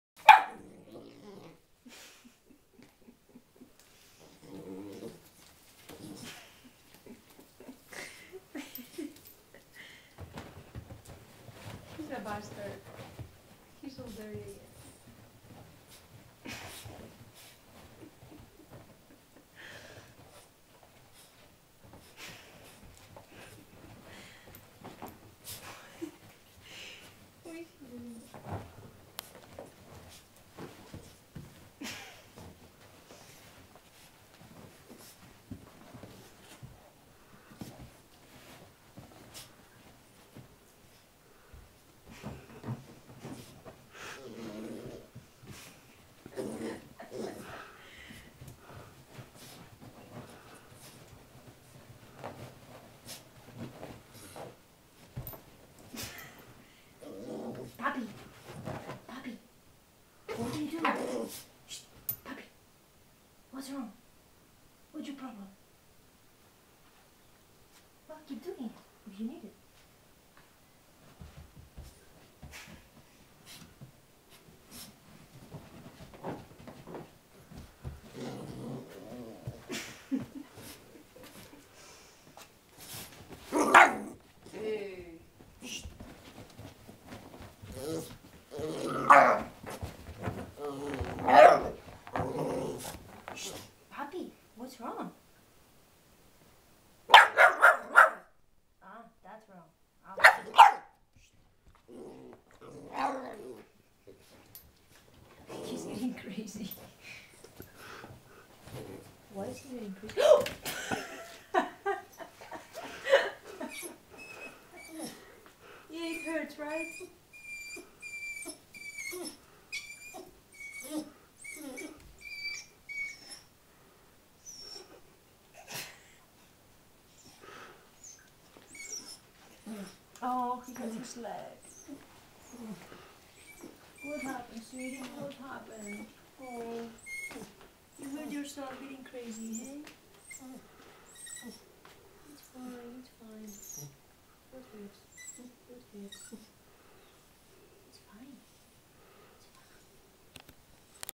Wet puppy goes loco and injures himself.
He just got his first bath and really wanted to dry off...